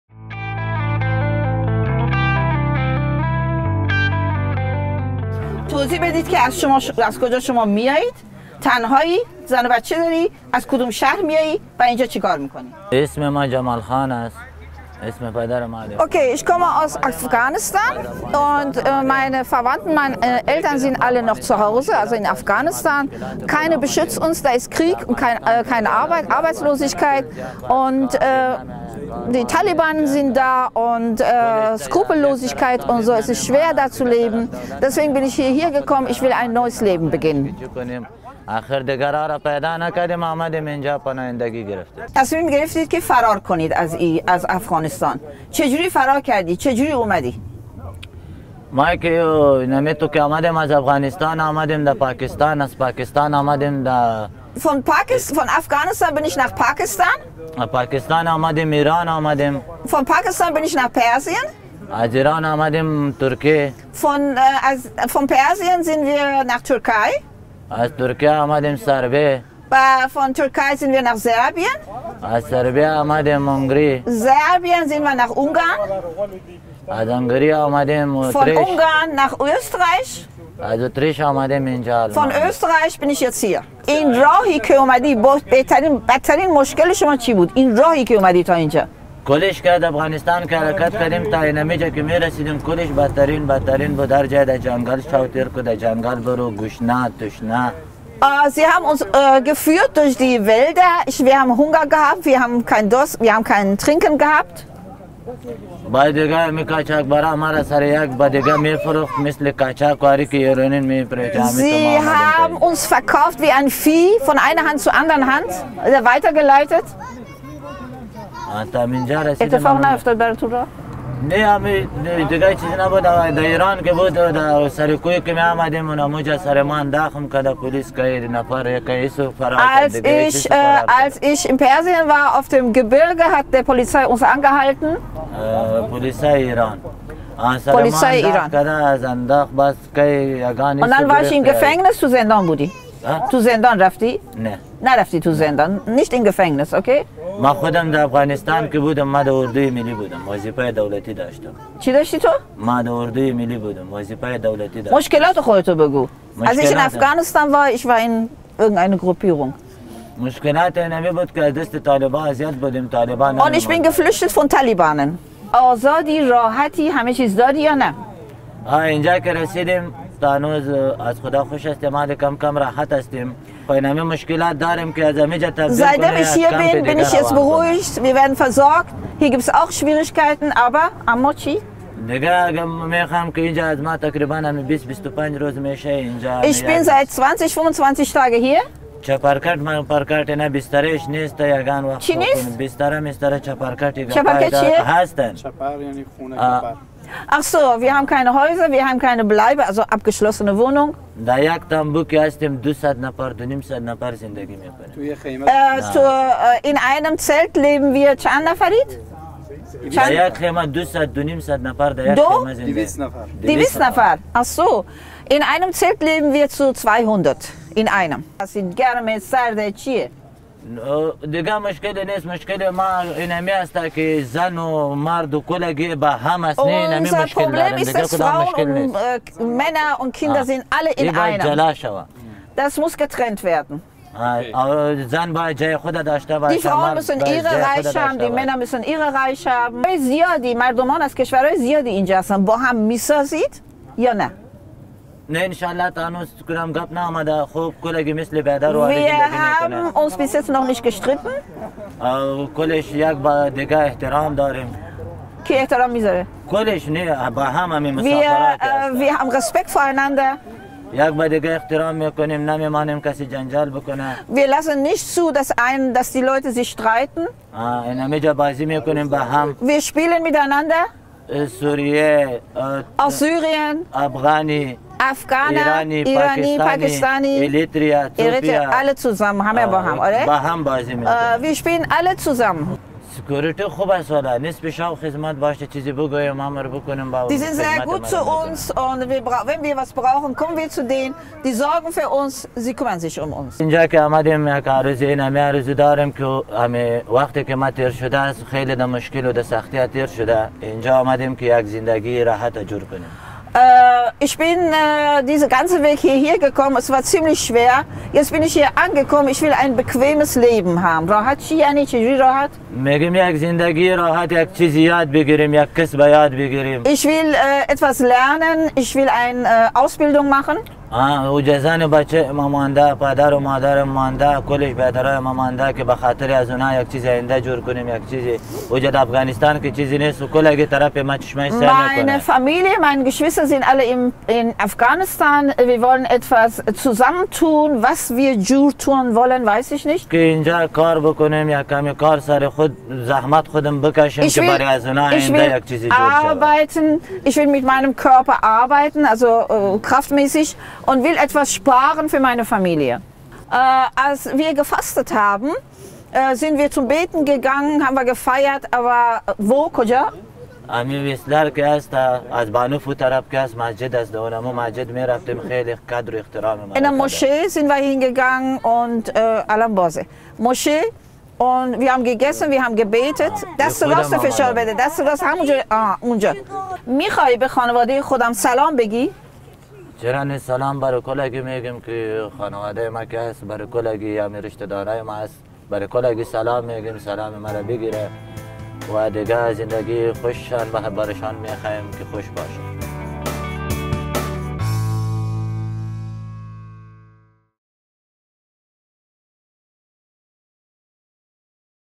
Woher kommst Du - Wohin gehst Du? - Jamal
Jamal aus Afghanistan

Videoprojekt mit Flüchtlingen aus dem Wetzlarer Zeltcamp
Woher kommst Du? Warum hast Du Deine Heimat verlassen? Wie ist die Situation im Zeltcamp? Welche Pläne hast Du?

Viele Fragen an die meist jungen Flüchtlinge. Jedes Interview wird zu einer intensiven Begegnung. Wir erfahren über das Schicksal eines Menschen und unser Gegenüber fühlt sich ernst genommen als Neuankömmling, als Nachbar*’in und Mitbewohner*’ – als Mensch.

Die Gespräche und Interviews werden auf unserem Youtube Kanal und auf einer extra eingerichteten Facebook-Projektseite zu sehen sein.